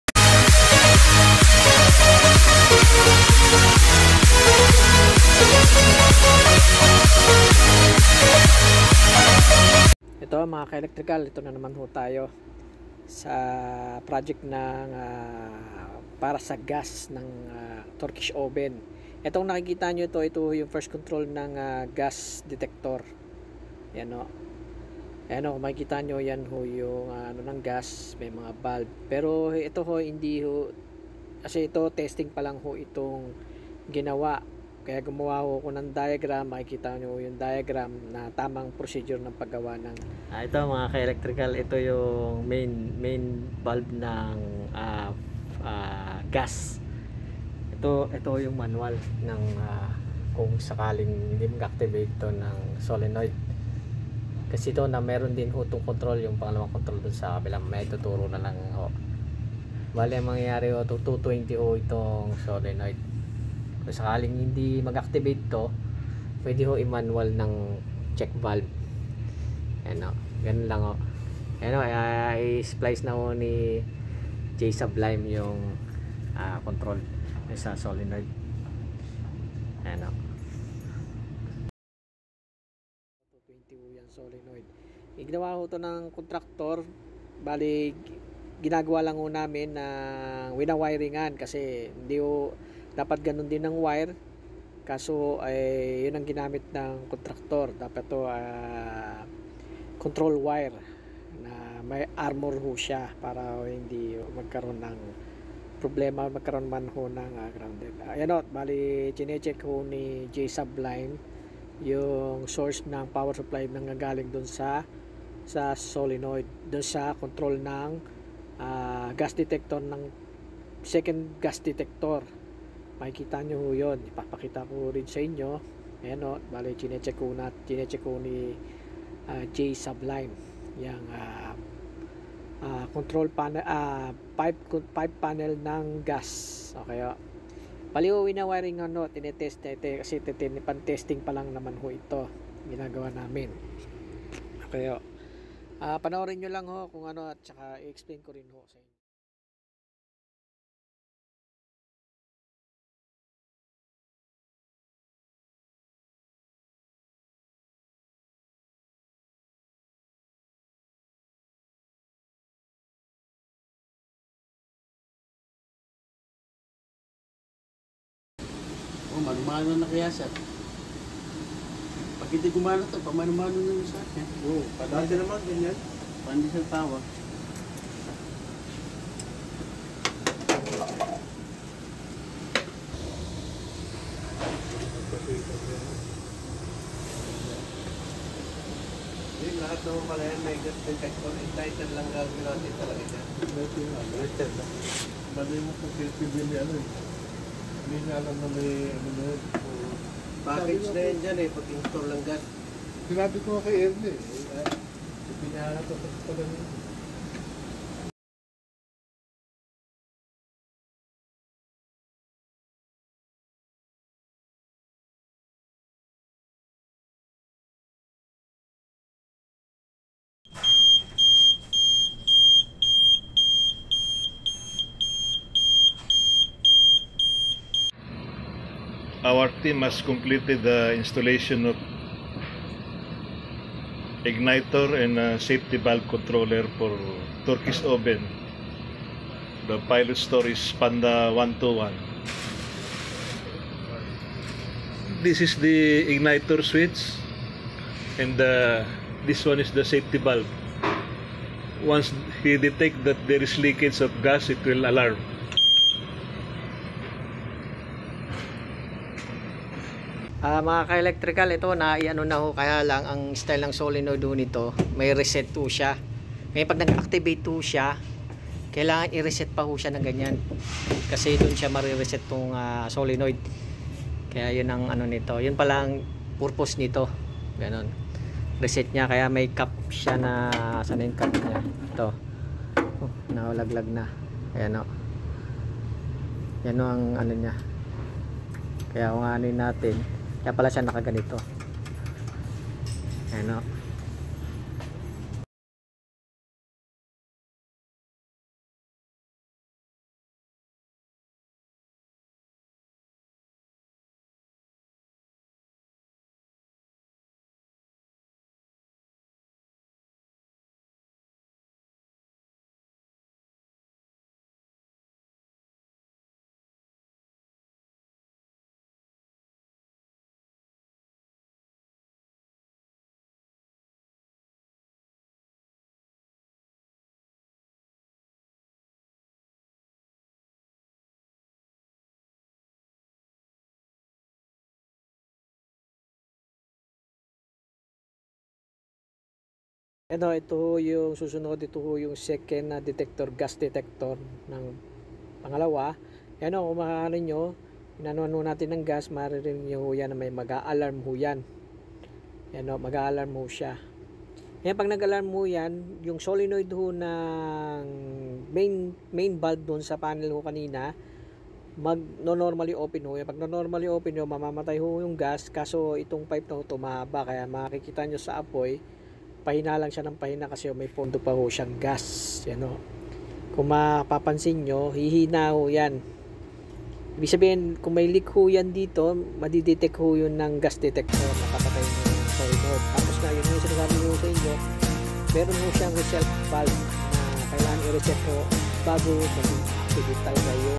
GAS DETECTOR model SE230KG
installing for gas detector in turkish oven ..( TECNOCONTROL MODEL no: SE230KG GAS DETECTOR)